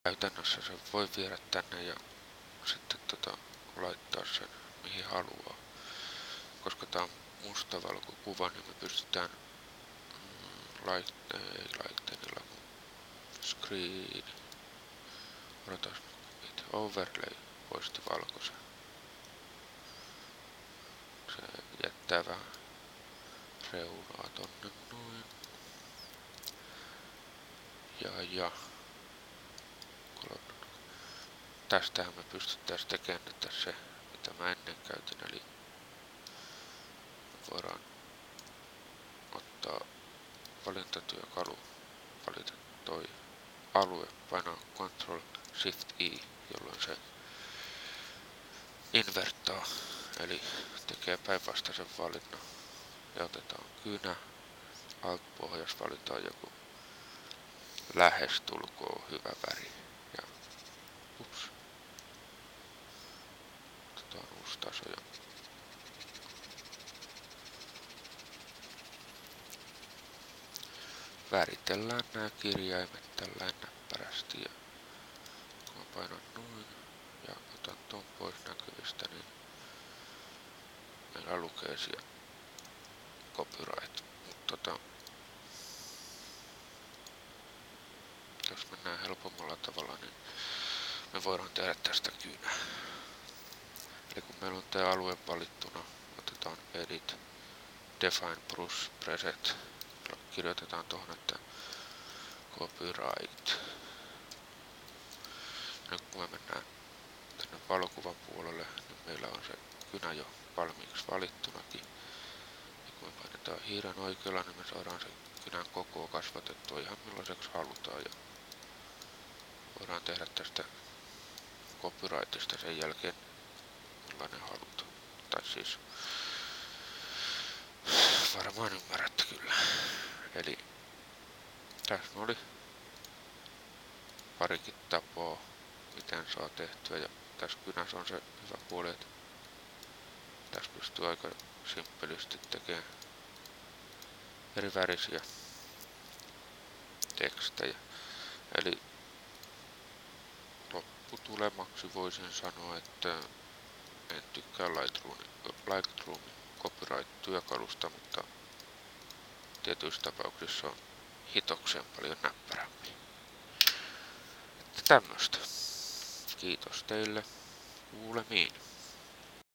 1. Vesileiman teko Lightroom & Photoshop 2/2
Kuinka tehdä vesileima lightroomiin ja käyttää sitä sekä vesileiman teko Photoshopilla.